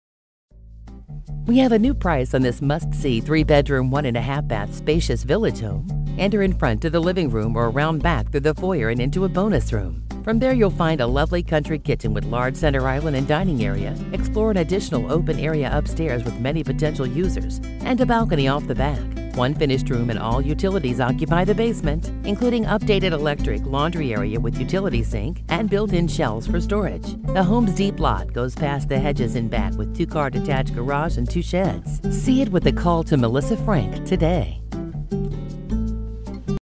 84 Smith St Concord, NY Homes for Sale | wnymetro.com
84 Smith St, Concord, NY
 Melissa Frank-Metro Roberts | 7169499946

New price on this MUST SEE 3 Bdrm, 1.5 bath spacious Village home!  Entry in front to living rm or back to foyer into bonus rm, country kitchen w/ large center island and dining area.  Upstairs has additional open area w/ many uses and balcony off back.  Large full, updated bath and 3 spacious bedrooms with good closet space.  Basement has one finished room, all utilities are there, furnace new in '95, updated electric, laundry area w/ utility sink and built in shelve area for storage.  Deep lot goes past hedges in back w/ 2 car det gar, patio area and 2 sheds, one with electric!